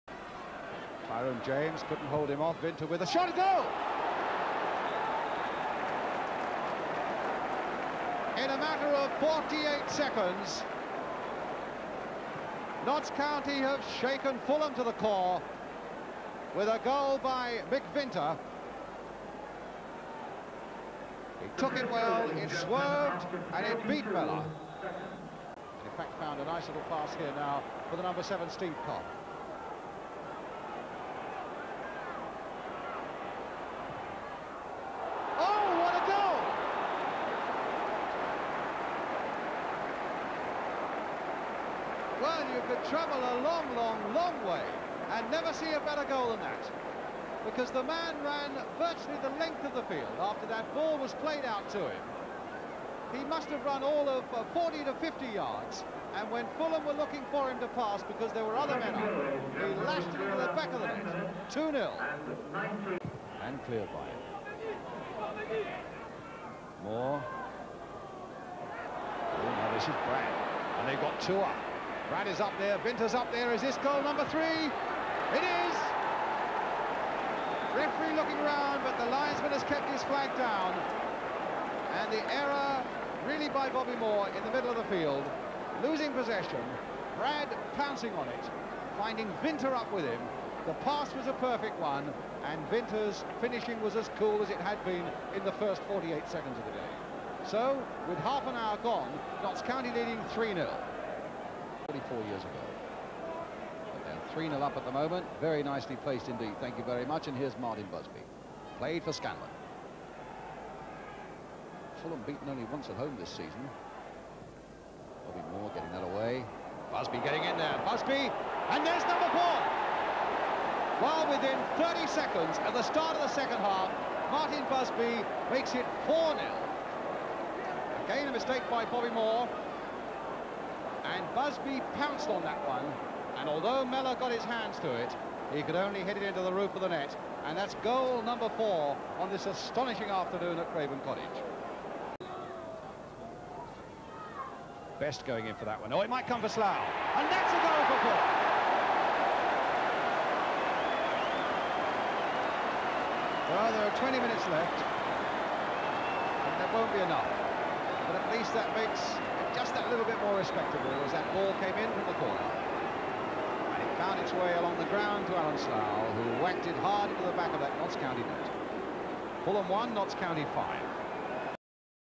Fulham v Notts County Division Two 20-11-1976
The idea of packing the Fulham team with fading stars like Moore, Best and Marsh isn't working too well. Fulham only have four wins so far. One County goal missing from these highlights.